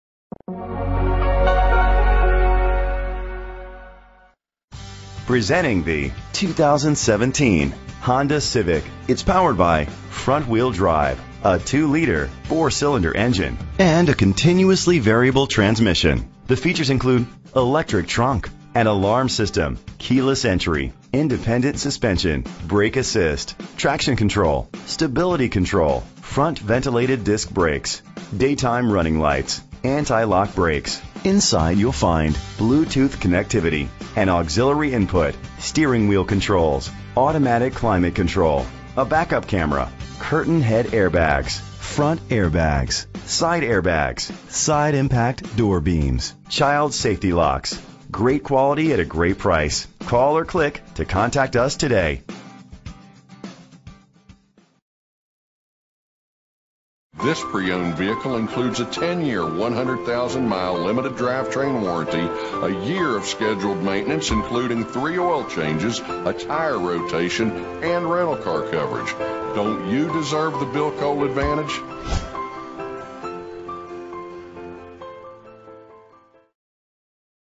2017 Honda Civic H3665 - Bluefield WV
Year: 2017
Make: Honda
Model: Civic
Trim: LX
Engine: 2.0 liter 4 Cylindercylinder FWD
Transmission: CVT
Color: Dk. Gray
Mileage: 1
Address:
VIN:2HGFC2F56HH506330